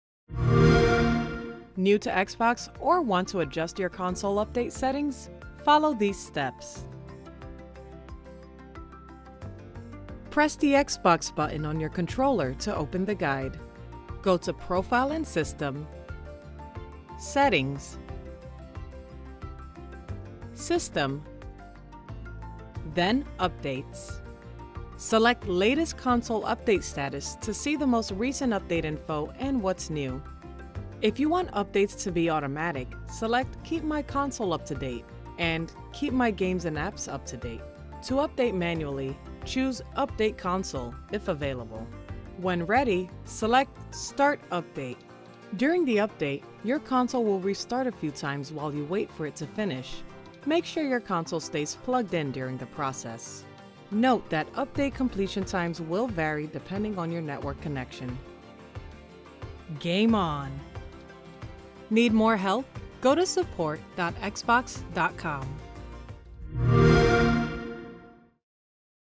Getting Started with Update Settings on Your Xbox Console
Follow these steps for options to keep your console and games to date